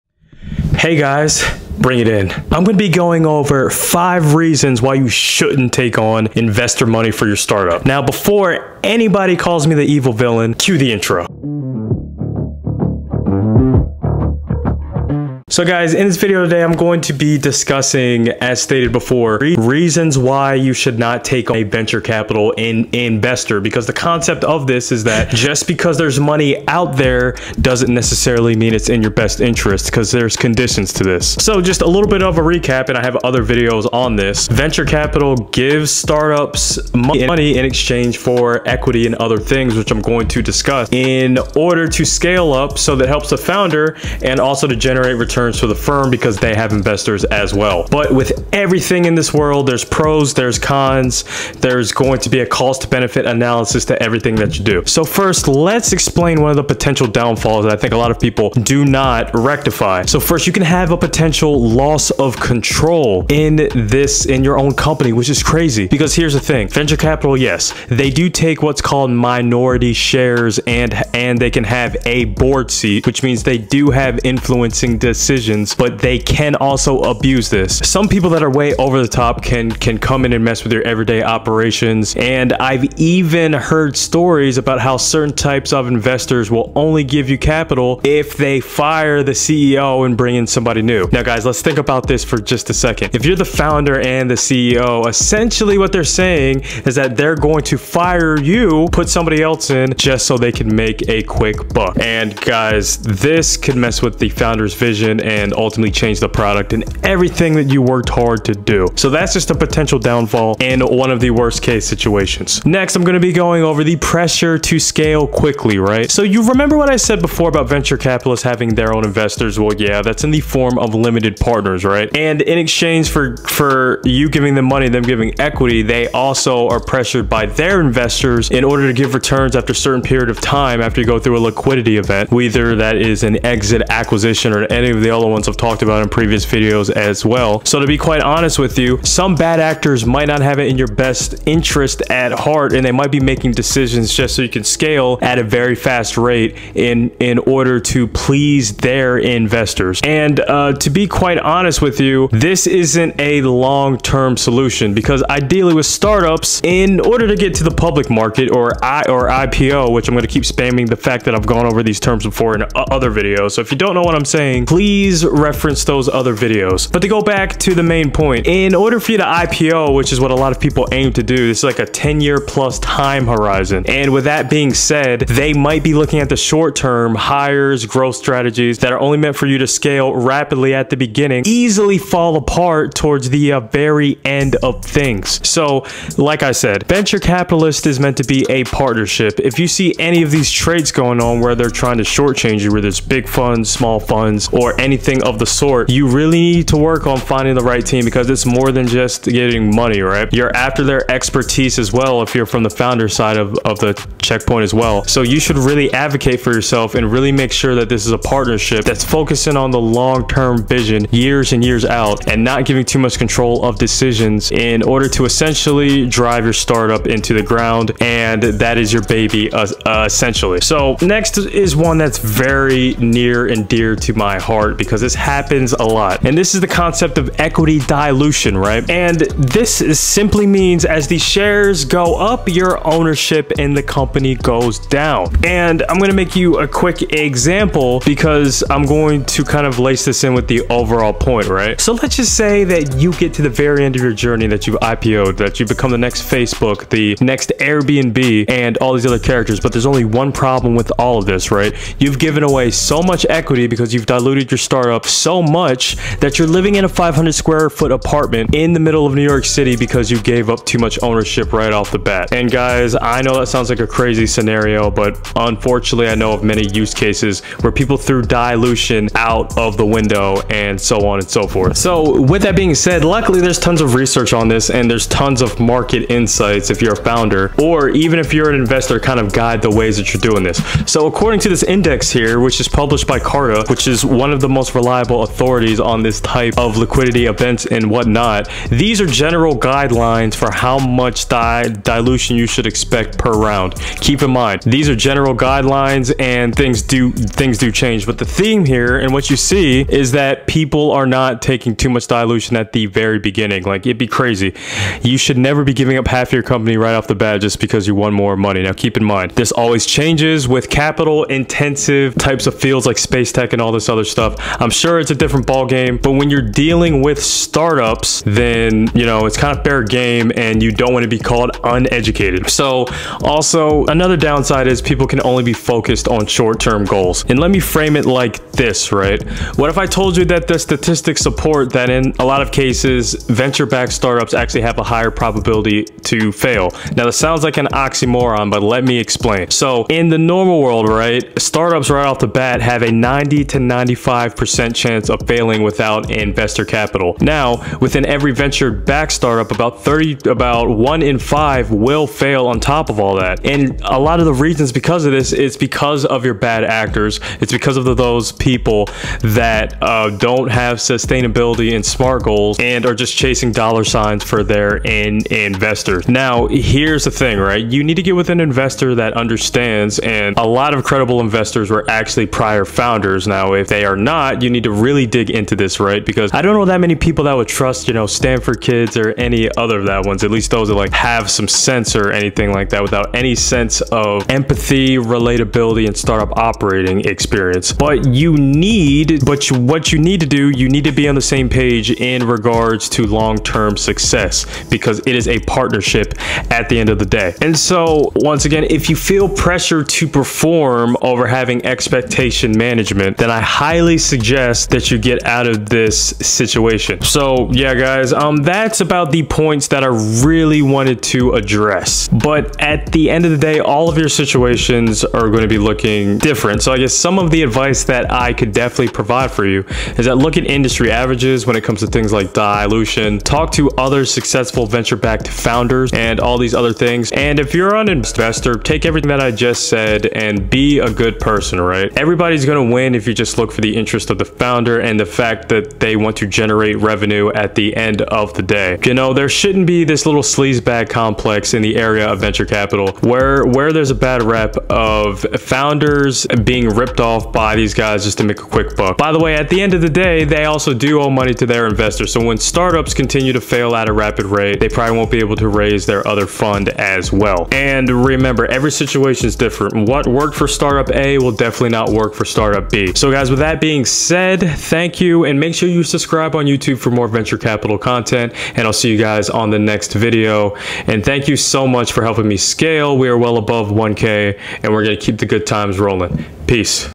5 Reasons NOT To Take VC Money
I discuss how taking Venture Capital Funding (capital in exchange for equity and expertise) may not be the best option for every startup